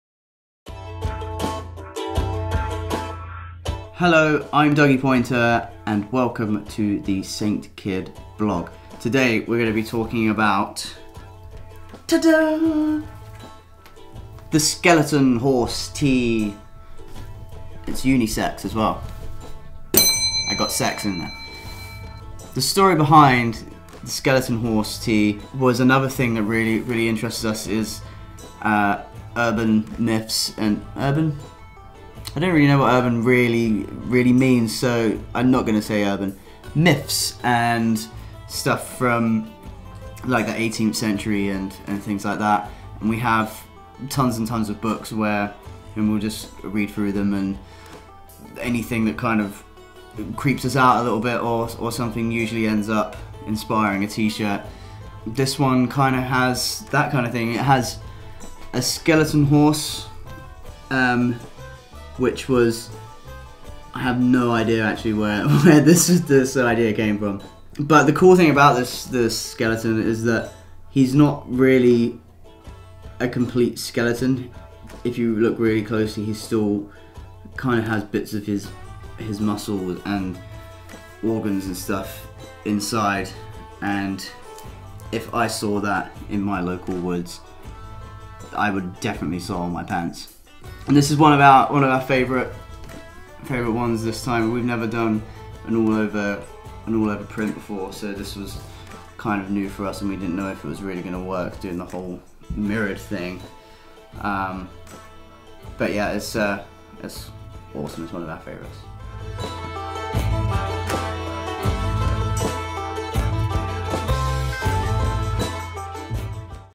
The Captain and The Skeleton Horse
The Captain, Dougie Poynter, talks through the Saint Kidd Skeleton Horse Tee.

You can see more like this on SaintKidd.com/Blog